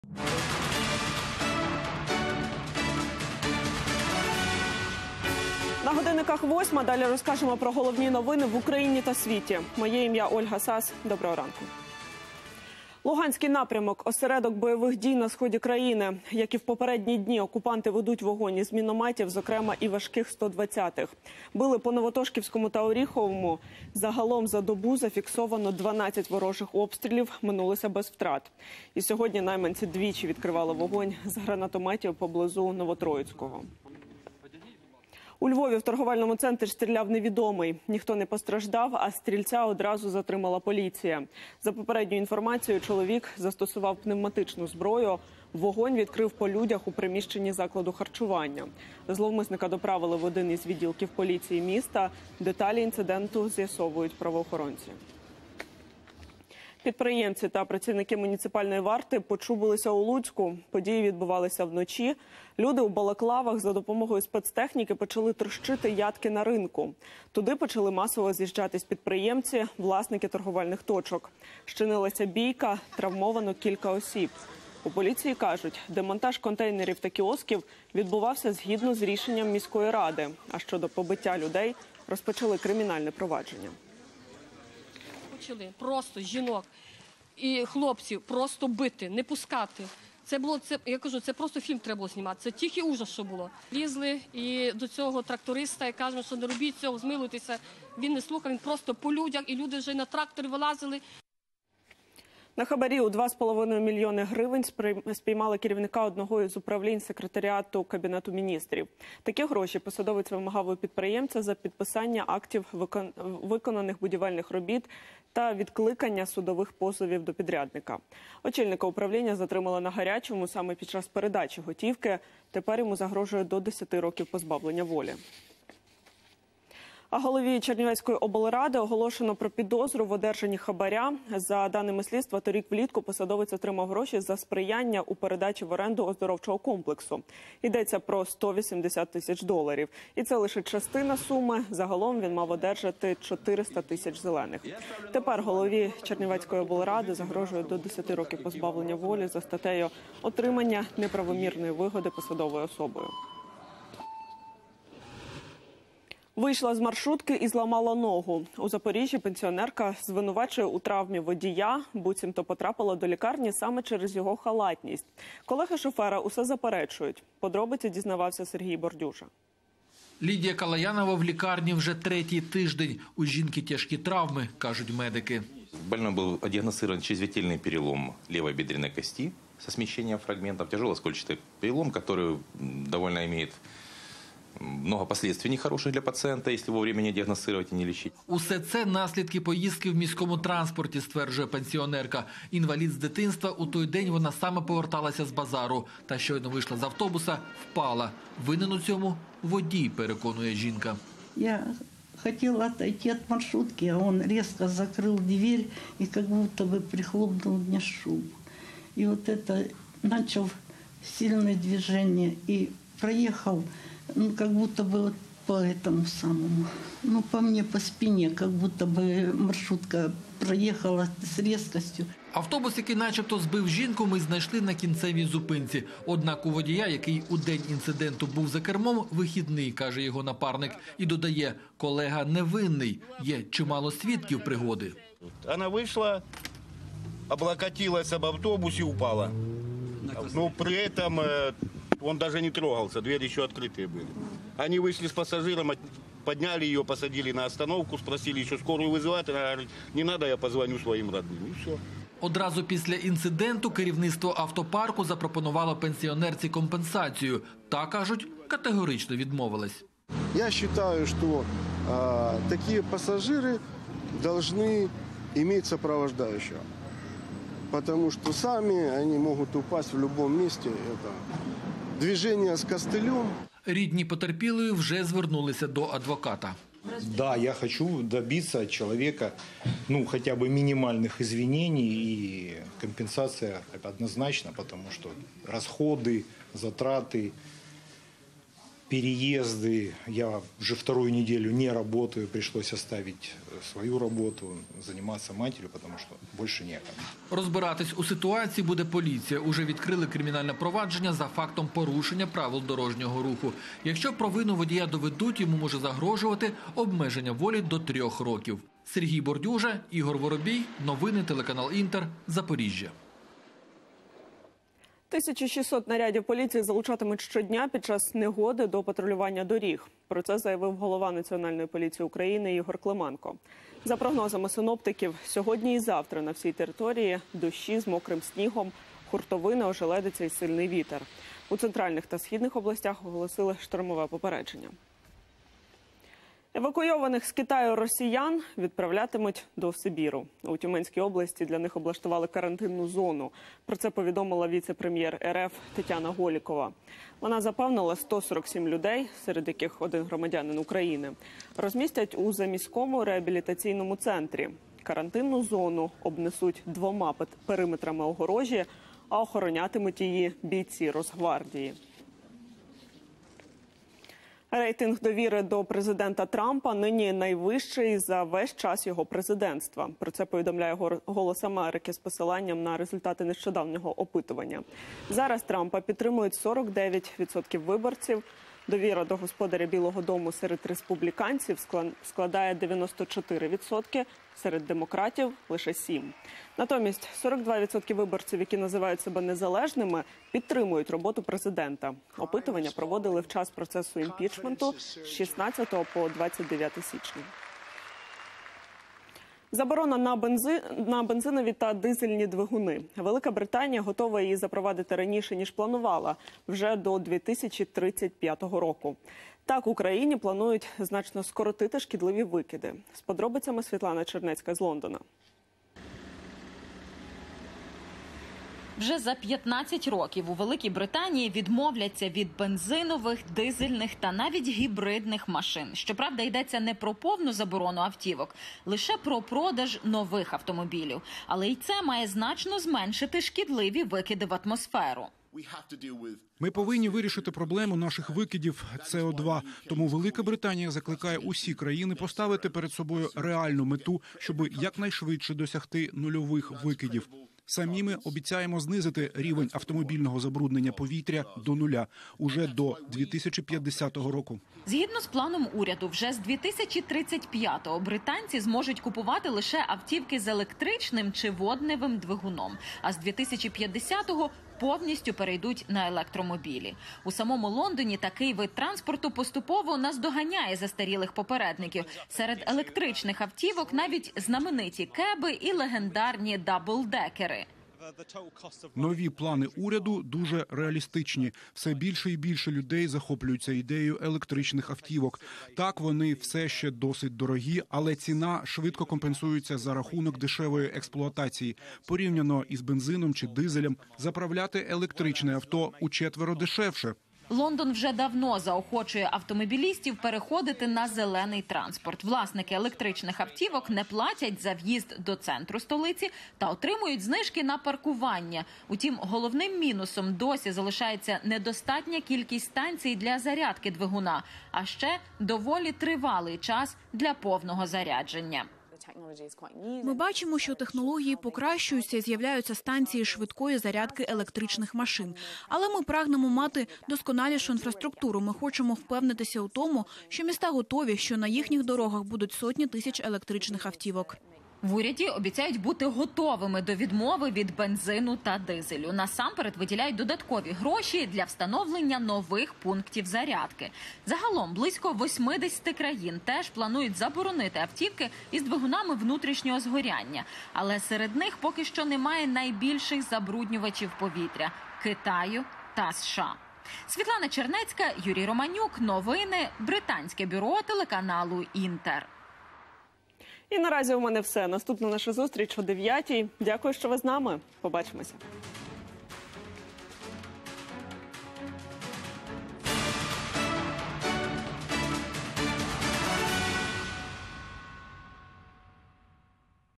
Новости 8:00, выпуск за 5 февраля 2020 года
Дивіться у випуску: Голові Чернівецької облради оголошено про підозру в одержанні хабаря. У Луцьку люди у балаклавах потрощили ятки на ринку. Евакуйованих з Китаю росіян відправлять до Сибіру